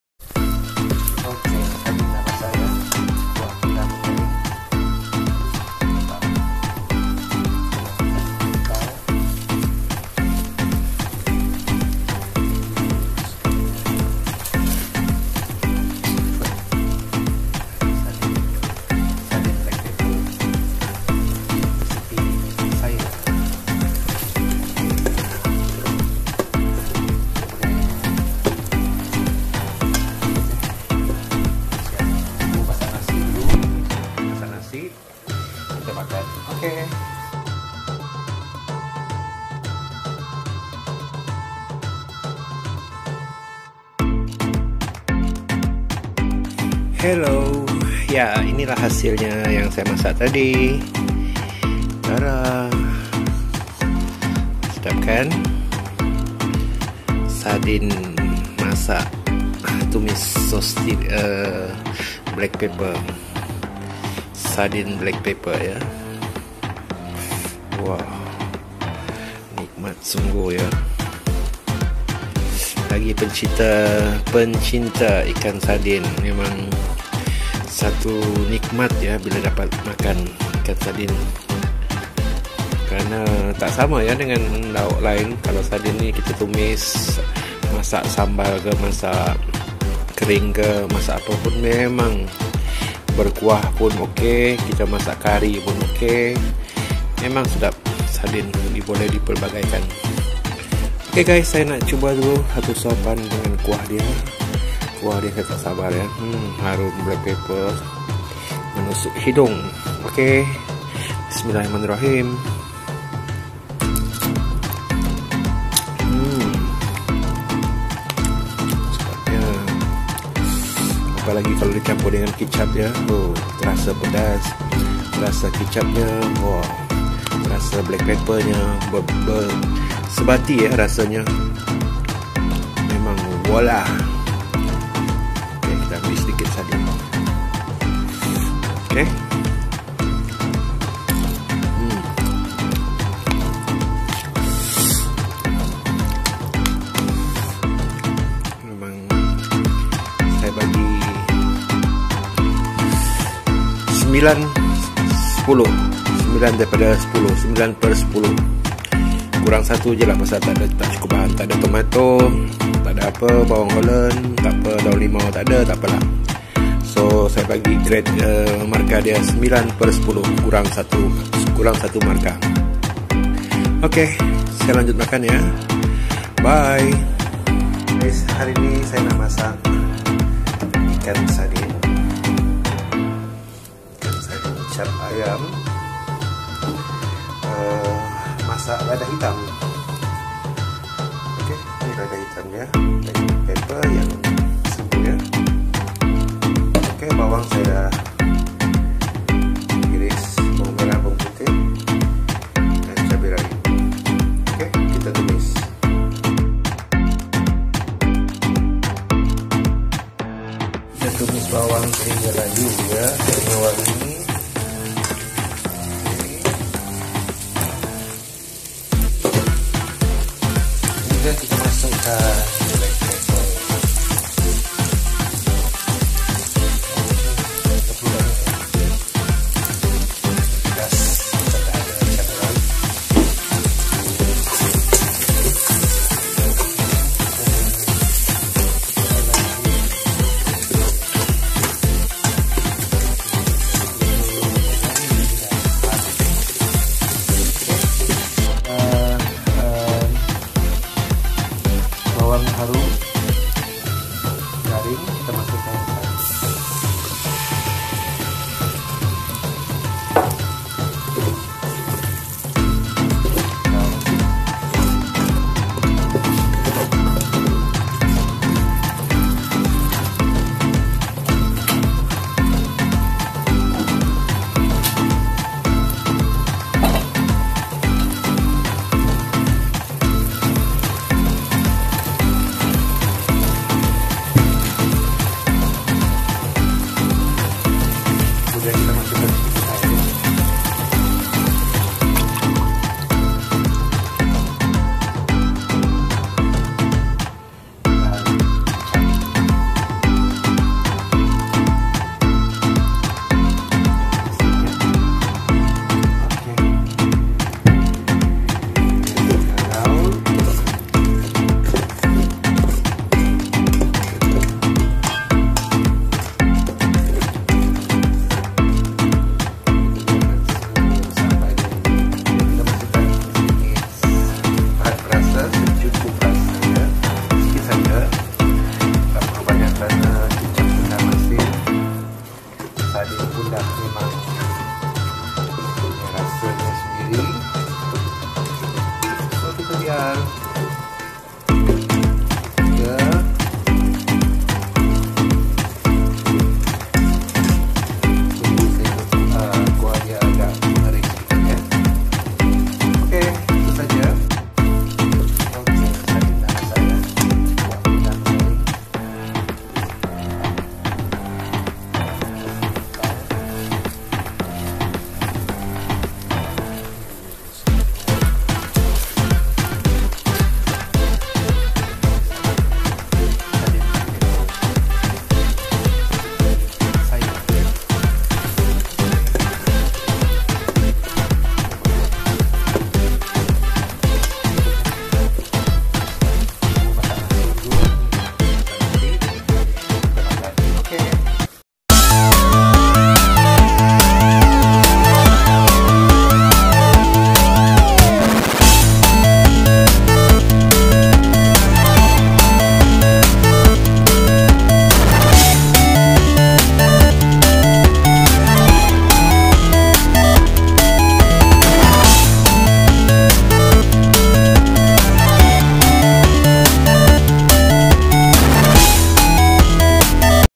V211 Sardin Black Pepper. Resepi cara masak ikan sardin yang sedap dan simple
Cara masak ikan sardin black pepper yang mudah dan sedap.